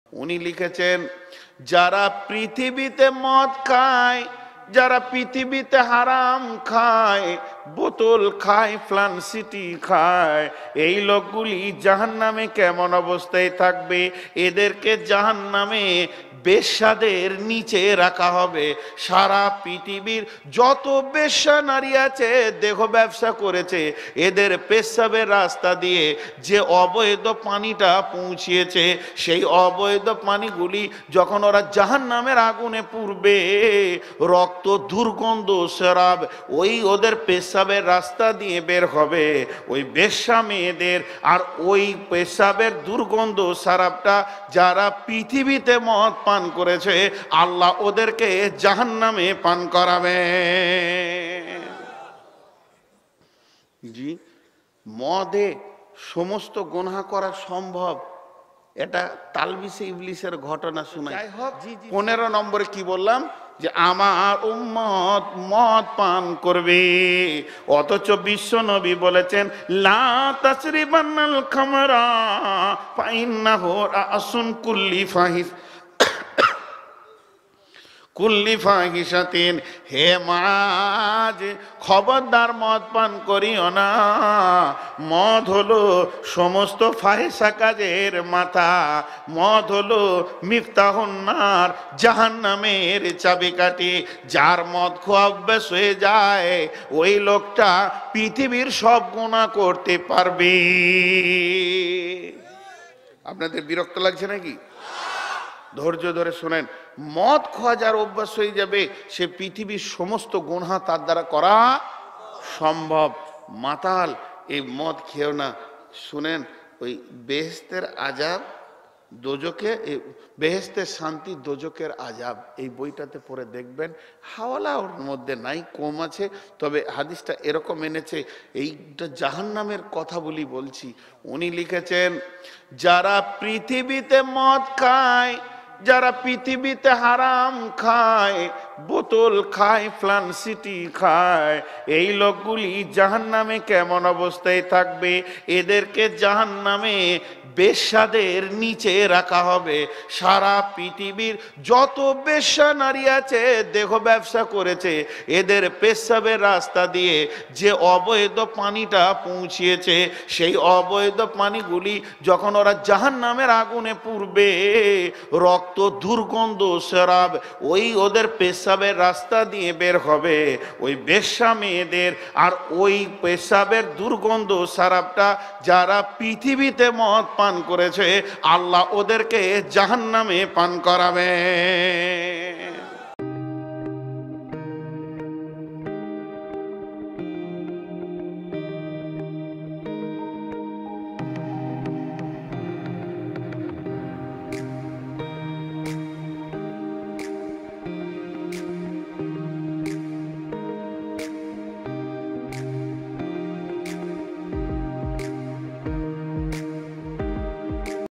যারা মদপান করে জাহান্নামে তাদের শাস্তি যেমন হবে।শাইখ নুর মুহাম্মদ বর্ধমানী।
যারা মদপান করে জাহান্নামে তাদের শাস্তি কেমন হবে আলোচনায় শাইখ নুর মুহাম্মদ বর্ধমানী।nur mohammad bardhamani waz new waz .